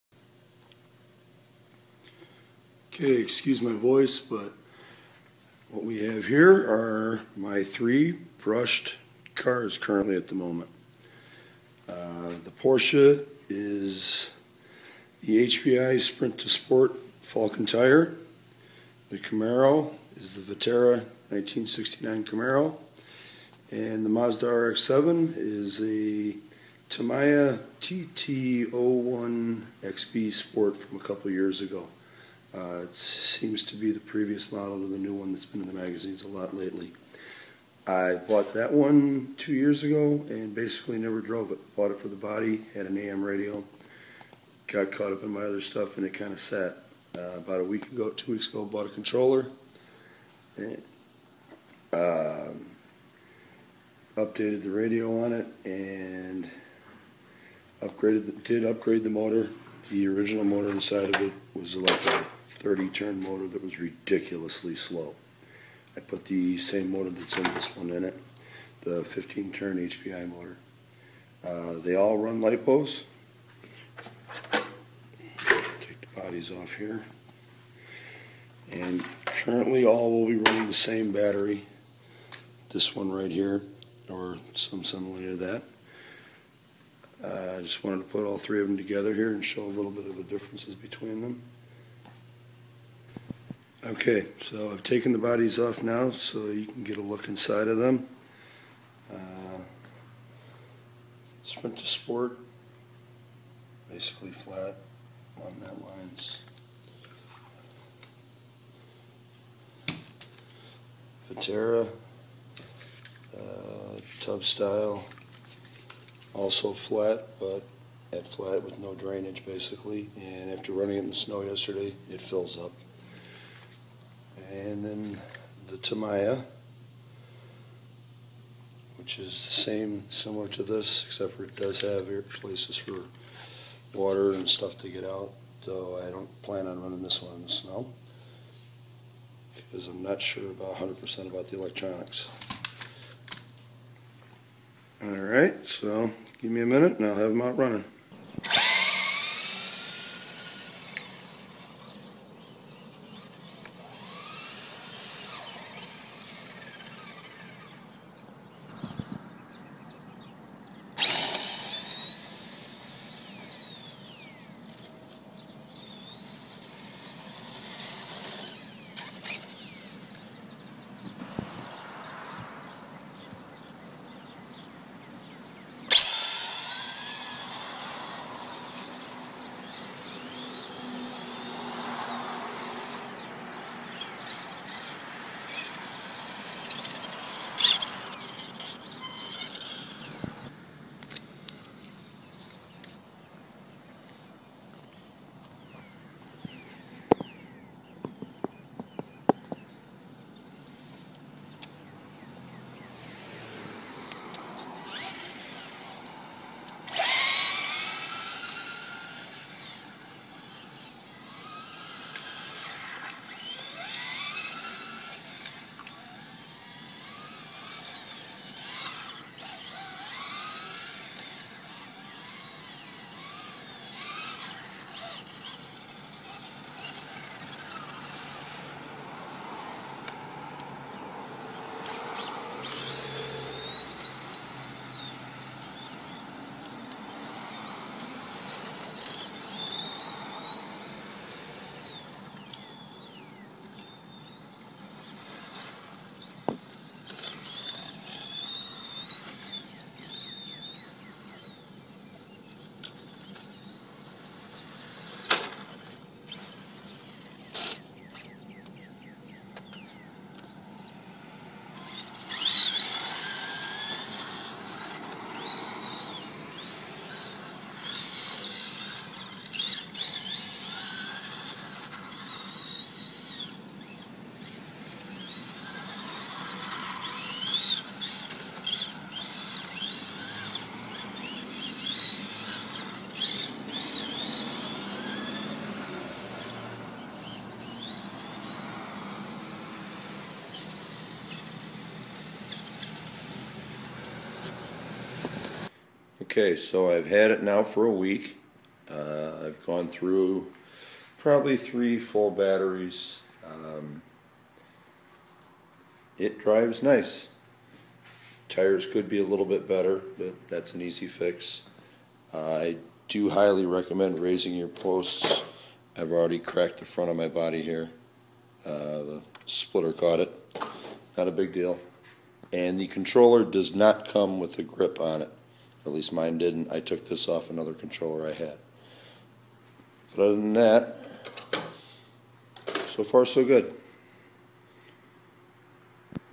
Hpi. Vaterra. Tamiya. On road
Sprint 2 Sport. Vaterra. Tamiya TT-01. All on 2s 45c batteries. Just wanted to show 3 different versions of brushed cars. Hpi is a bit quicker and faster.